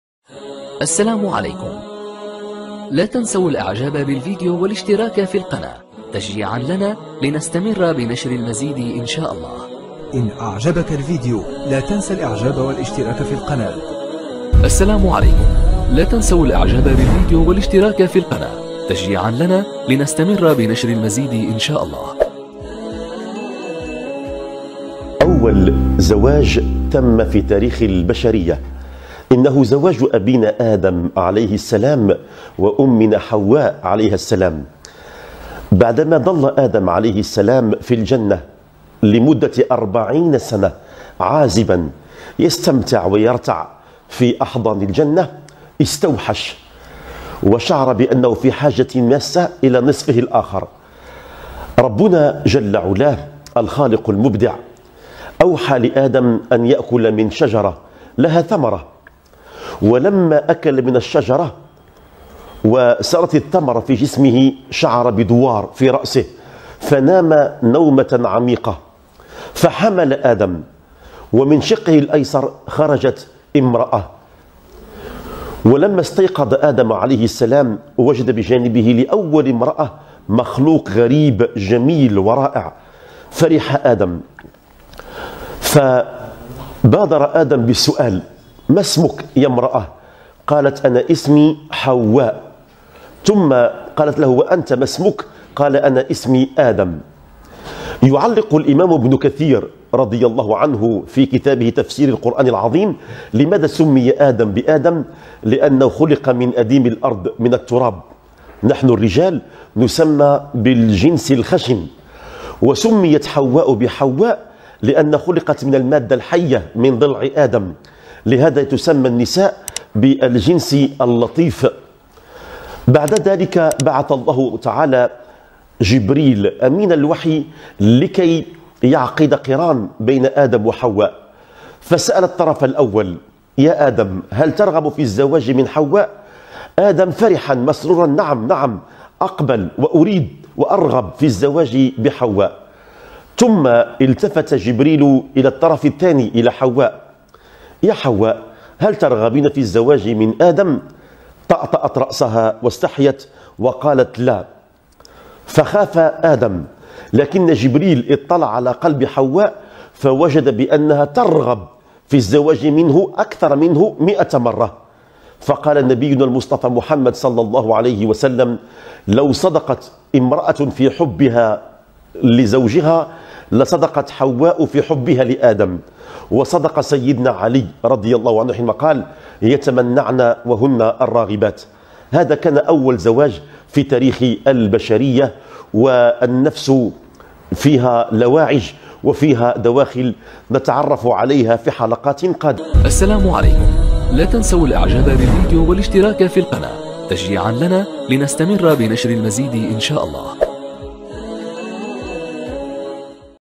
ماهو أول زواج في تاريخ البشرية �� الدكتور طيب كريبان
أول زواج في تاريخ البشرية 🕌 الدكتور طيب كريبان 👉 دين و دنيا
⛔ أحبابي الكرام ، إذا أعجبكم الفيديو رجاءاً لا تنسوا :
 ▬▬▬▬▬▬ஜ۩☺۩ஜ▬▬▬▬▬
مواضيع سابقة
 ▬▬▬▬▬▬ஜ۩☺۩ஜ▬▬▬▬▬
 ▬▬▬▬▬▬ஜ۩☺۩ஜ▬▬▬▬▬
 ▬▬▬▬▬▬ஜ۩☺۩ஜ▬▬▬▬▬
⚠️ توضيح مهم ❗

 ▬▬▬▬▬▬ஜ۩☺۩ஜ▬▬▬▬▬
# هاشتاغ الموضوع #